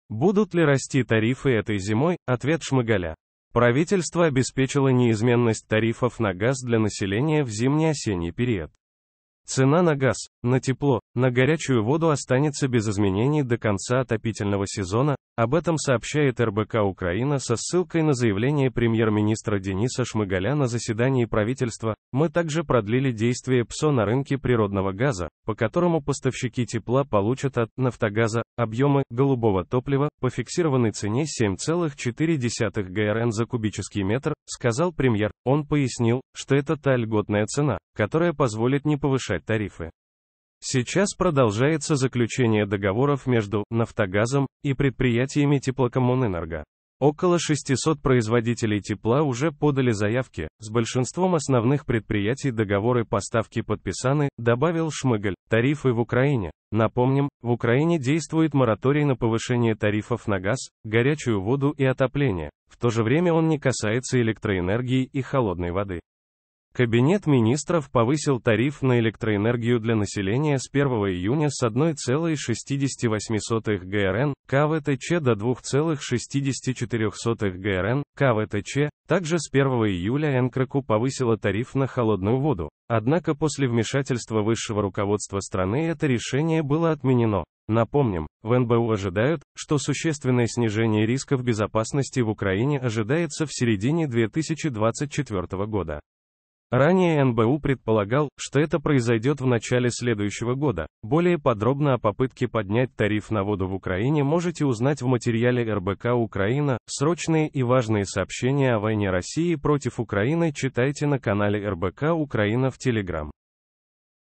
Будут ли расти тарифы этой зимой: ответ Шмыгаля.
Премьер-министр ДенисШмыгаль Правительство обеспечило неизменность тарифов на газ для населения в зимне-осенний период. Цена на газ, на тепло, на горячую воду останется без изменений до конца отопительного сезона. Об этом сообщает РБК-Украина со ссылкой на заявление премьер-министра Дениса Шмыгаля на заседании правительства. "Мы также продлили действие ПСО на рынке природного газа, по которому поставщики тепла получат от "Нафтогаза" объемы "голубого топлива" по фиксированной цене - 7,4 грн за кубический метр", - сказал премье